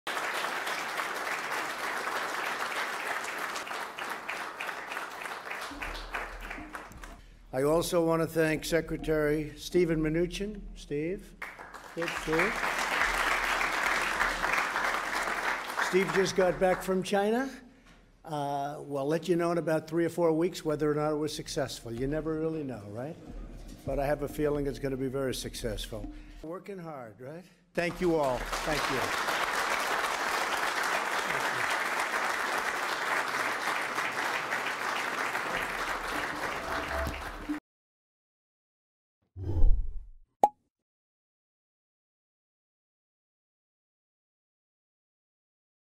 트럼프 "중국과 무역협상 성공 예감…3∼4주내 알게 될 것" / 연합뉴스 (Yonhapnews)
(서울=연합뉴스) 도널드 트럼프 미국 대통령은 중국과의 무역협상이 성공할지 여부를 앞으로 3∼4주일 내에 알게 될 것이라며 성공할 것이라는 느낌이 든다고 13일(현지시간) 밝혔다.
트럼프 대통령은 이날 백악관 만찬에서 스티븐 므누신 재무장관이 2주일 전 중국에서 무역협상을 마치고 돌아왔다는 점을 지적하면서 이렇게 말했다.
그는 "그(므누신 재무장관)가 막 중국에서 돌아왔다. 우리는 그것이 성공적이었는지 아닌지를 3∼4주일 내에 여러분들에게 알려줄 것"이라면서 "하지만 나는 그것이 매우 성공할 것이라는 예감을 갖고 있다"고 설명했다.

(영상 : 로이터)